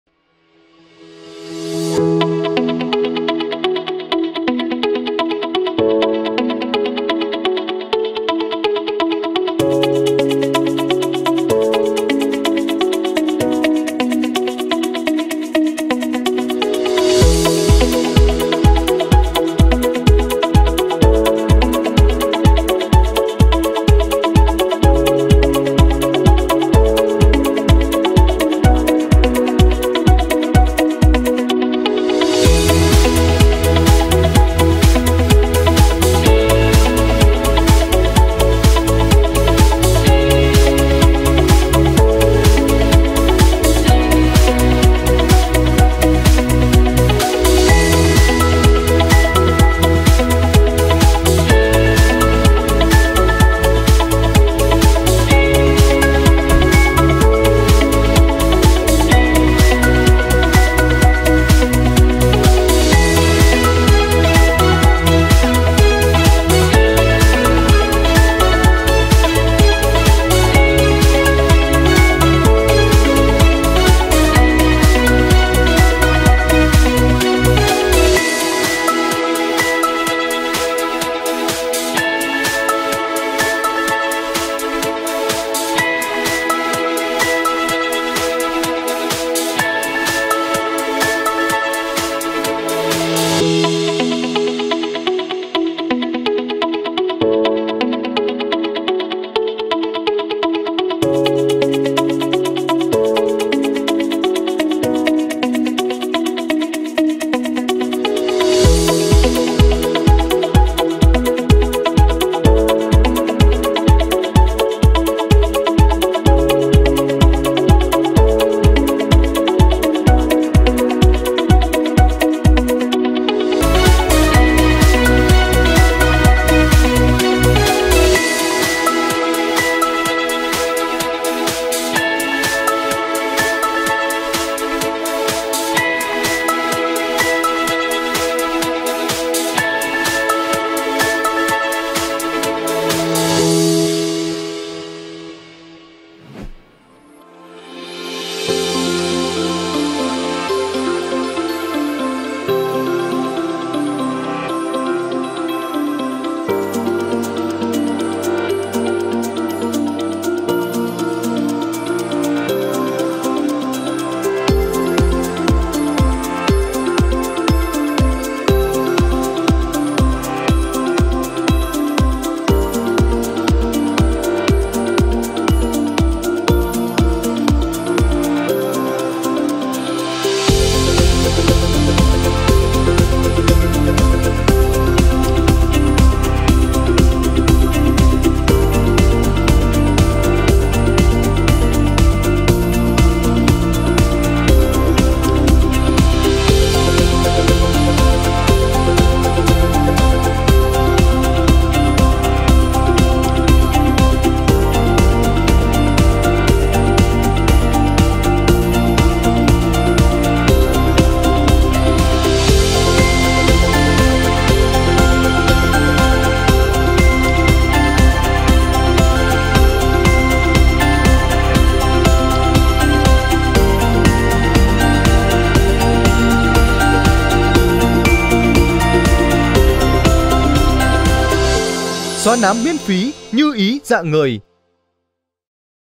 Highlight: Sự Kiện Xóa Nám Miễn Phí - Tập Đoàn Themphaco
Chào mừng quý khách đến với video highlight của sự kiện Xóa Nám Miễn Phí, tổ chức bởi Tập Đoàn Themphaco tại Trung Tâm Hội Nghị Công Đoàn Việt Nam.

Trải qua một ngày sôi động và ý nghĩa, sự kiện đã thu hút hơn 1000 người tham dự, tại đây hàng trăm khách hàng đã được trải nghiệm liệu trình xóa nám không xâm lấn - một công nghệ tiên tiến và an toàn, đồng thời được nhận phần quà là bộ sản phẩm xóa nám trị giá 5 triệu đồng, mang lại niềm vui và sự tự tin cho mỗi người tham dự.

Không chỉ là một sự kiện làm đẹp thông thường, đây còn là cơ hội để gặp gỡ và trò chuyện với các chuyên gia hàng đầu từ Viện Đào Tạo và Nghiên Cứu Khoa Học Y Dược Quân Dân Y. Quý khách hàng đã có dịp được tư vấn và tìm hiểu rõ hơn về làm đẹp, chăm sóc da mặt từ những người có kiến thức và kinh nghiệm uy tín.

Sự kiện này là một phần của dự án Xóa Nám Miễn Phí cho 1 triệu người Việt, một nỗ lực mang lại sự tự tin và hạnh phúc cho cộng đồng. Và đừng lo lắng nếu bạn đã bỏ lỡ sự kiện này, vì các sự kiện tương tự sẽ tiếp tục diễn ra trên khắp cả nước trong thời gian tới.

Hãy cùng nhau chia sẻ niềm vui và sự tự tin từ những trải nghiệm đẹp tại sự kiện Xóa Nám Miễn Phí của Tập Đoàn Themphaco.

tham gia ngay link nhóm HỘI CHỊ EM XÓA NÁM MỤN KHÔNG XÂM LẤN